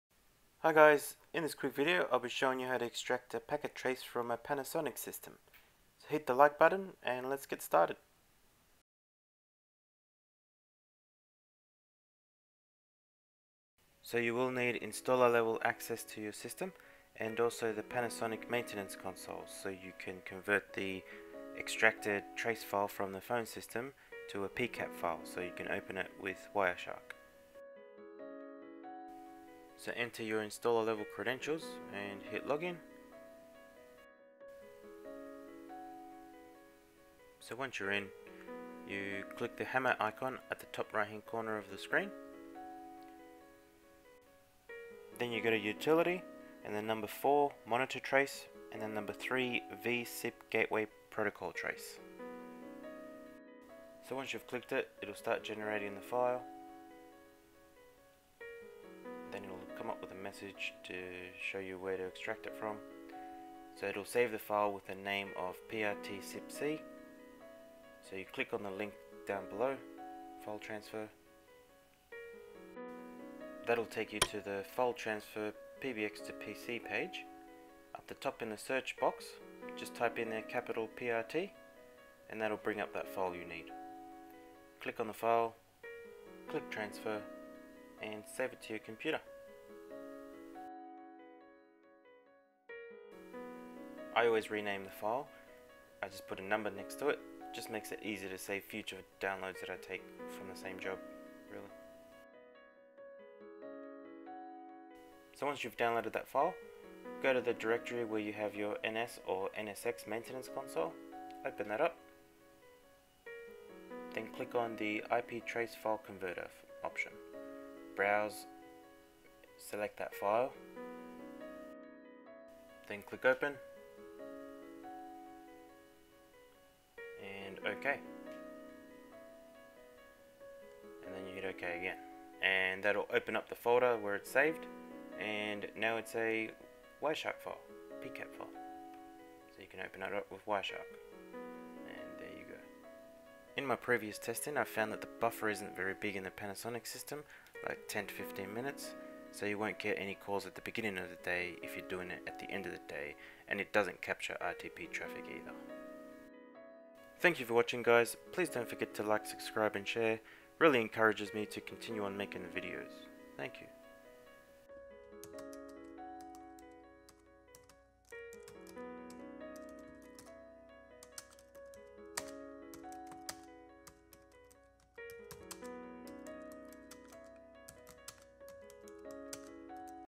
How to extract a sip packet trace from a Panasonic NS/ NSX System
This video is a quick guide on extracting a SIP/VoIP trace from the Panasonic KX-NS700, KX-NS100 and KX-NSX1000/2000 Phone systems.
You will need:

- Access to your phone system from your dealer
- NS / NSX unified maintenance console Software
- WireShark Software

External Mic